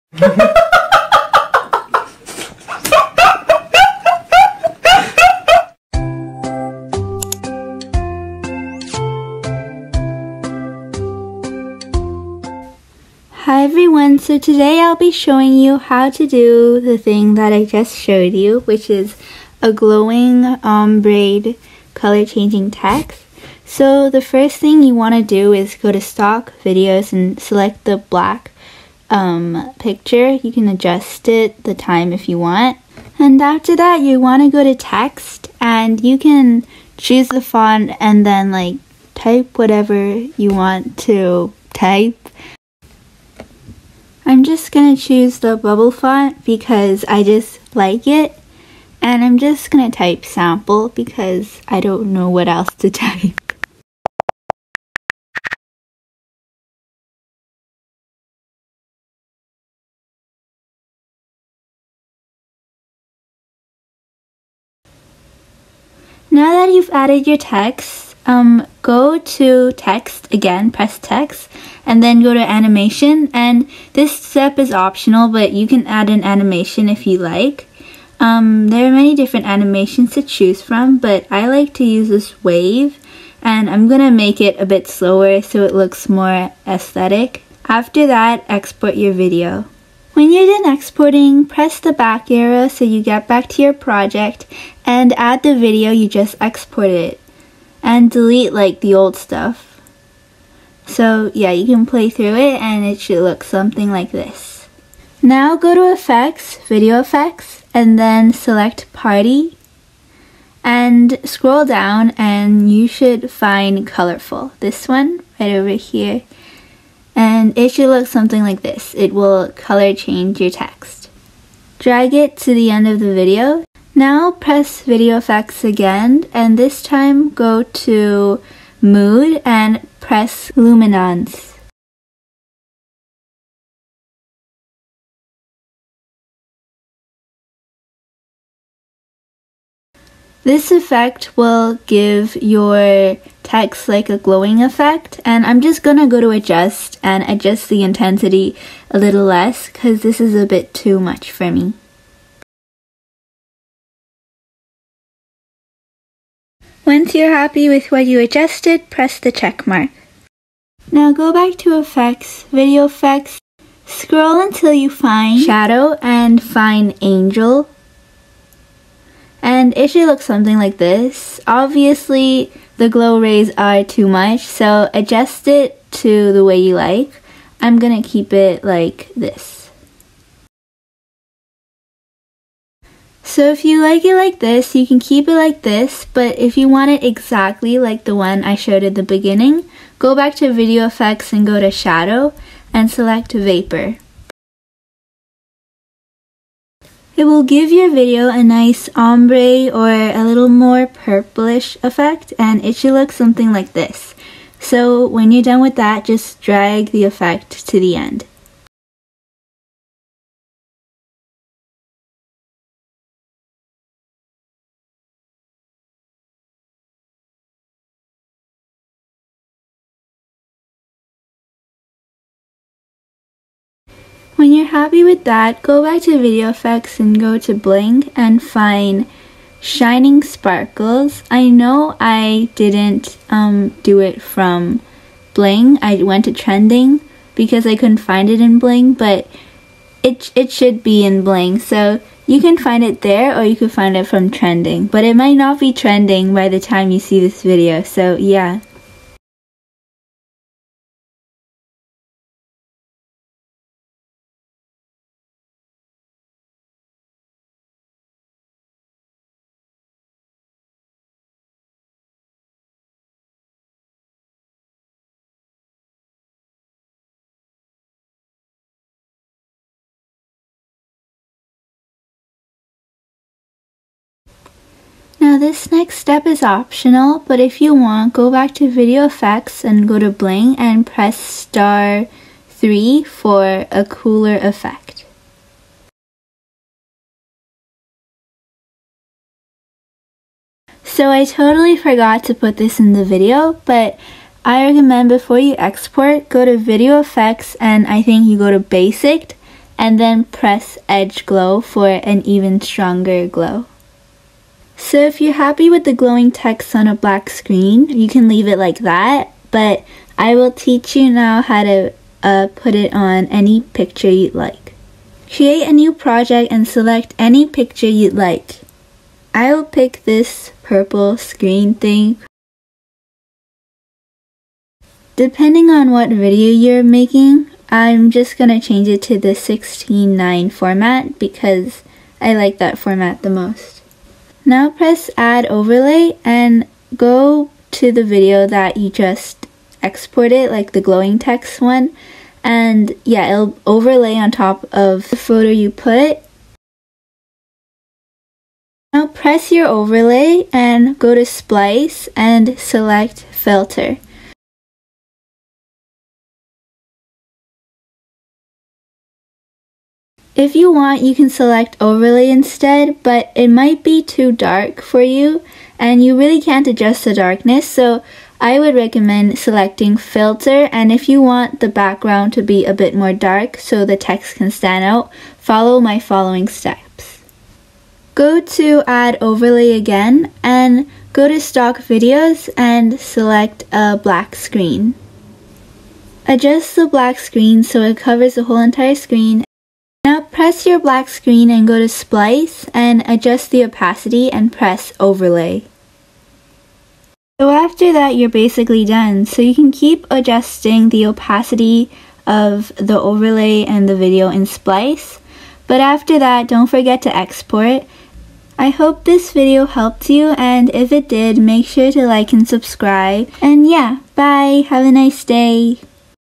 How to get glowing colour changing text on capcut!
Its my first tutorial so it might be confusing LOL anyways I hope it helped u enjoy luvs 💗